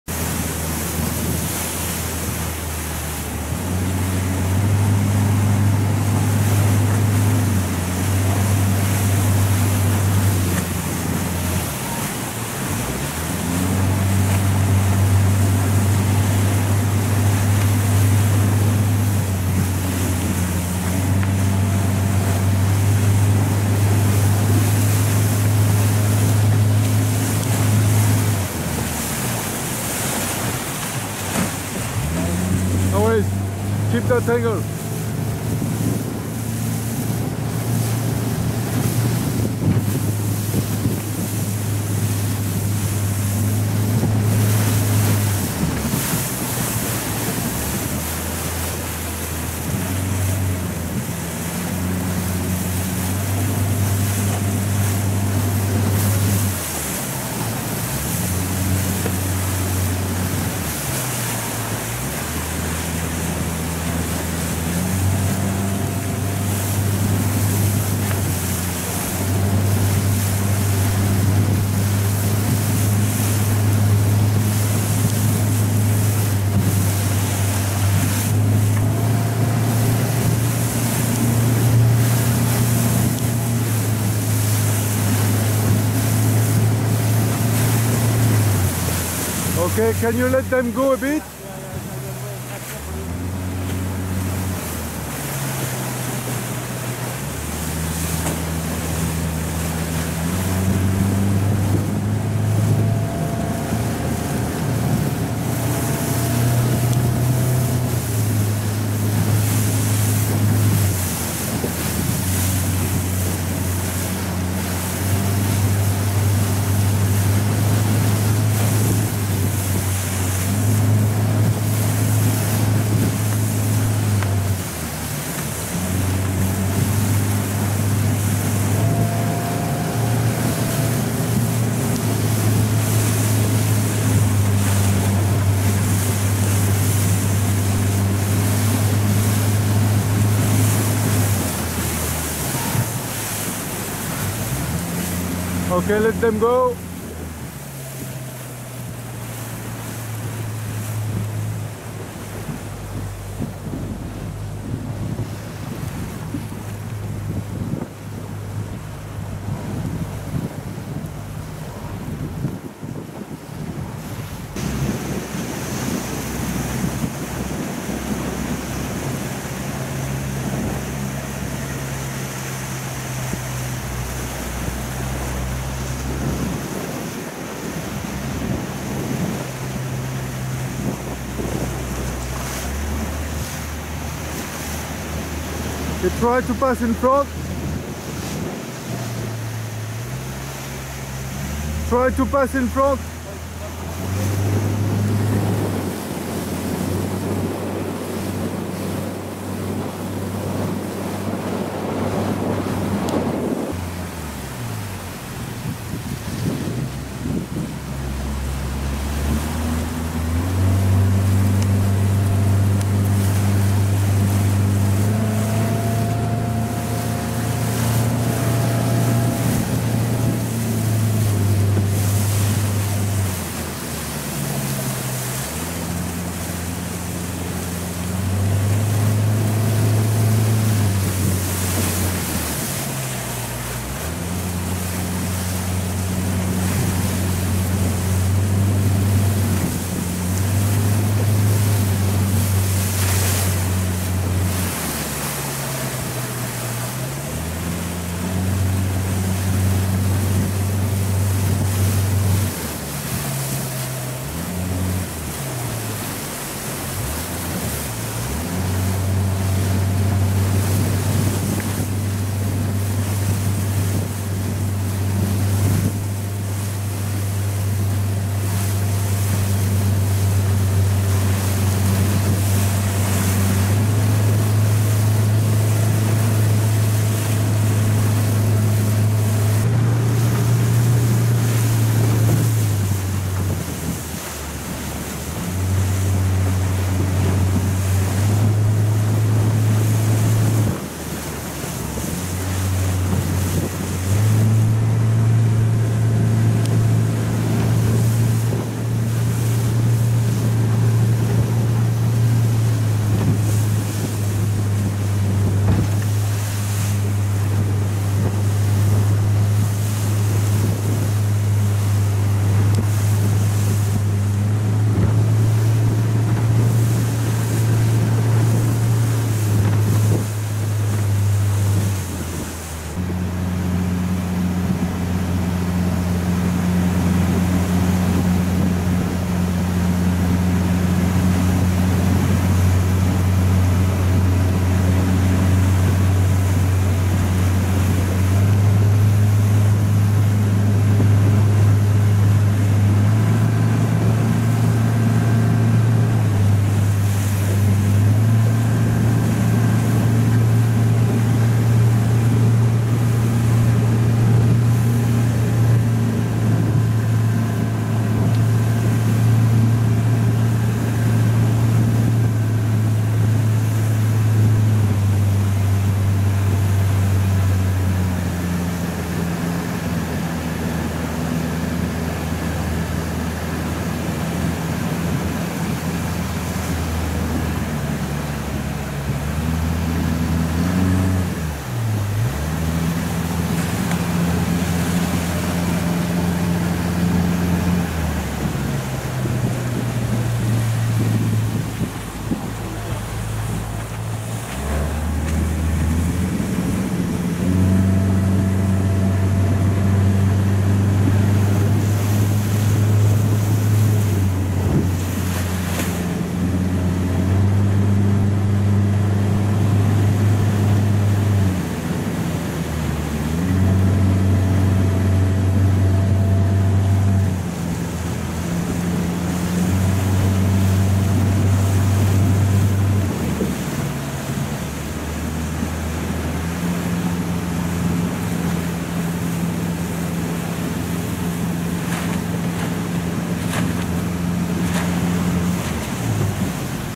505 CORK World Championships video from teams training prior to racing.
Some footage from teams out training on the the race course area for Cork World Championships